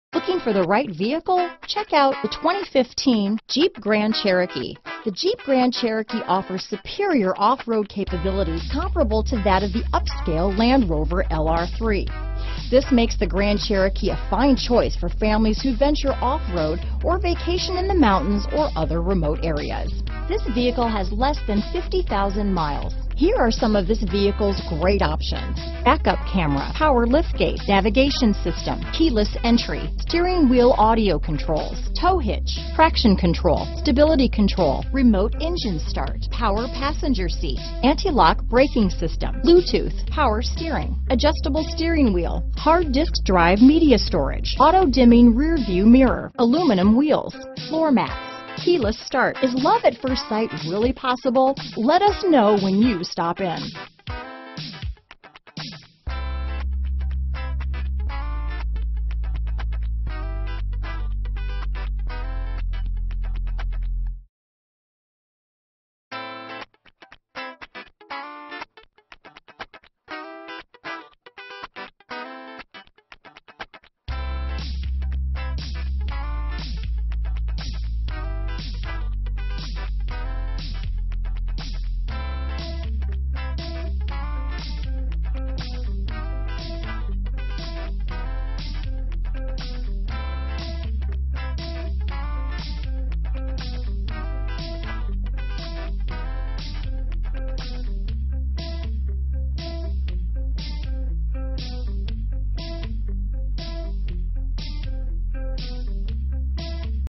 2015 Jeep Grand Cherokee Tulsa, Broken Arrow, Joplin, Bixby, Owasso, OK S19415A
Brilliant Black Crystal Pearlcoat Used 2015 Jeep Grand Cherokee available in Tulsa, Oklahoma at Matthews Ford. Servicing the Broken Arrow, Joplin, Bixby, Owasso, OK area.
2015 Jeep Grand Cherokee High Altitude - Stock#: S19415A - VIN#: 1C4RJFCGXFC110769

Matthews Ford
1101 Southwest Expressway Drive

Black 2015 Jeep Grand Cherokee 4WD 8-Speed Automatic 3.6L V6 24V VVT 4WD.Recent Arrival! Odometer is 17398 miles below market average!Reviews:* Strong and fuel-efficient engine lineup, including diesel V6; upscale interior; plenty of luxury and technology-oriented features; irrefutable off-road ability; outlandish SRT model. Source: Edmunds